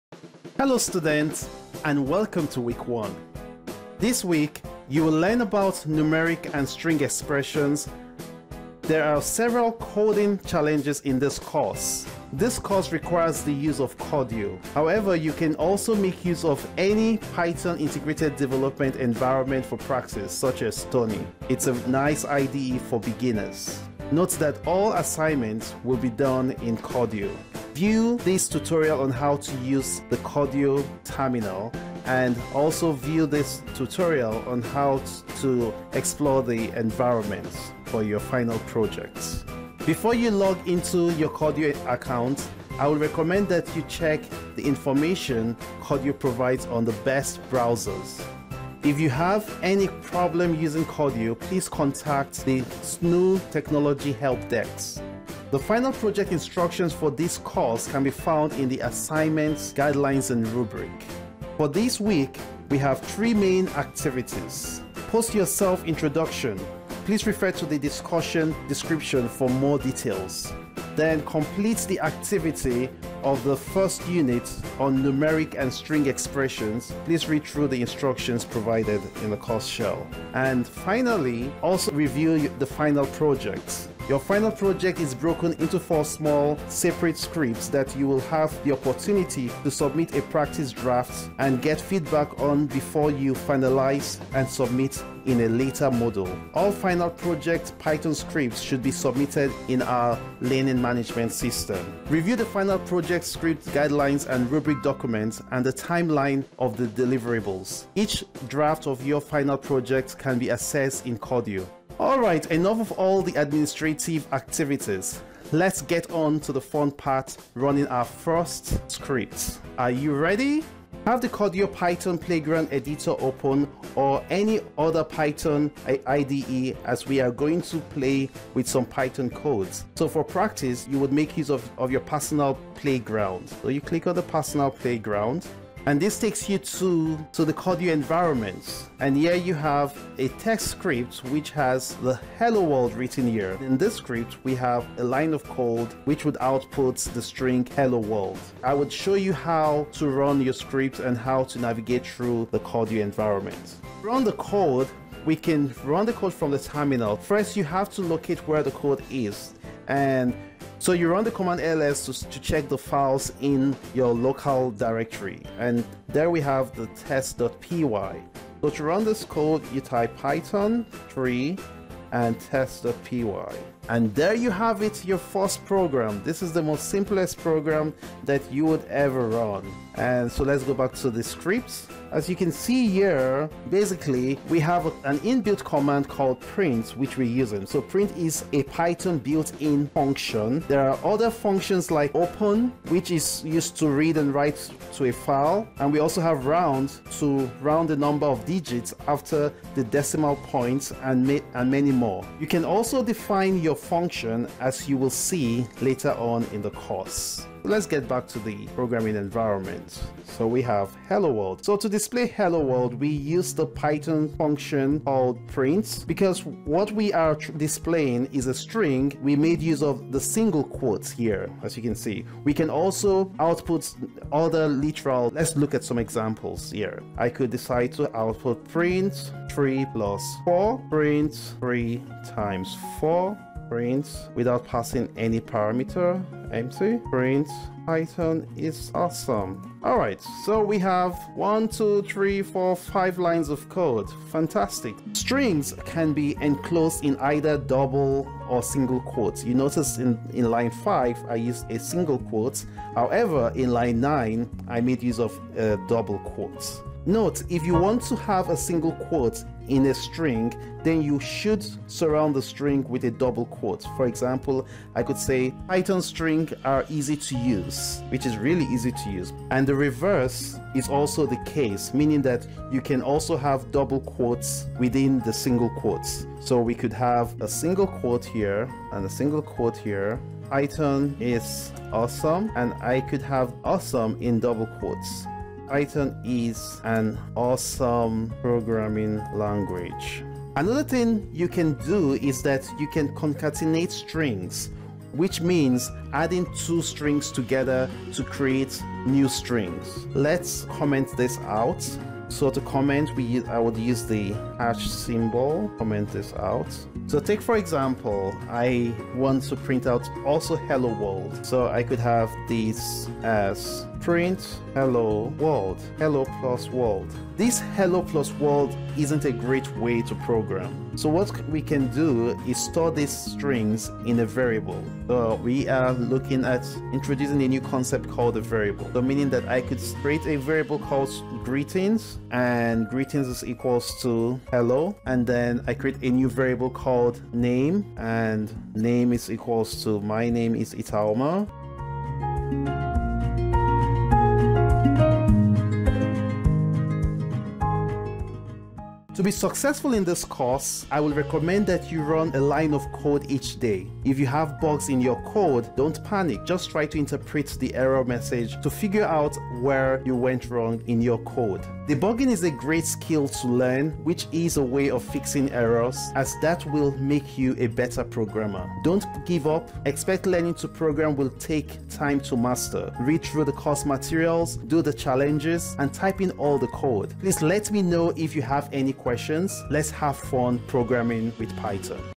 Week One: Numeric and String Expressions #1
Data and Expressions

Python is an Object oriented interpreted language and quite easy to use compared to other languages and runs on many OS such as Mac, Linux, and Windows.

Data in Python is strongly typed. If you add a number and a string, you will get an error.
It is also dynamically typed and you are free from worrying about variable declaration.

Despite the fact that it is easy, it is also very powerful.
It doesn't support backward compartibility. This means that some codes written in Python 2.X will not run in Python 3.x.

Have Codio Python Playground editor open or any other Python IDE as we are doing to play with some Python codes.

print('Hello, World!')

To display Hello, World! we used the Python print function. Because what we are displaying is a string, we made use of the single quotes. We can also output numerical numbers. For example,
print(1+2)
print(7*6)
print()
print("The End")

Another thing you can do is that you can concatenate strings. Which means adding two strings together to create a new string.
We can do that by using the + operator to join the strings or add the strings together.
print("hello" + " world")

This hello + world isn't really impressive. So, what we can do is store this strings in a variable.
greetings = "Hello"
name = 'Itauma'
print(greetings + name)
Another thing to do is adding a space between greetings and name
print(greetings + ' ' + name)

IT-140 @ SNHU
python tutorial for absolute beginners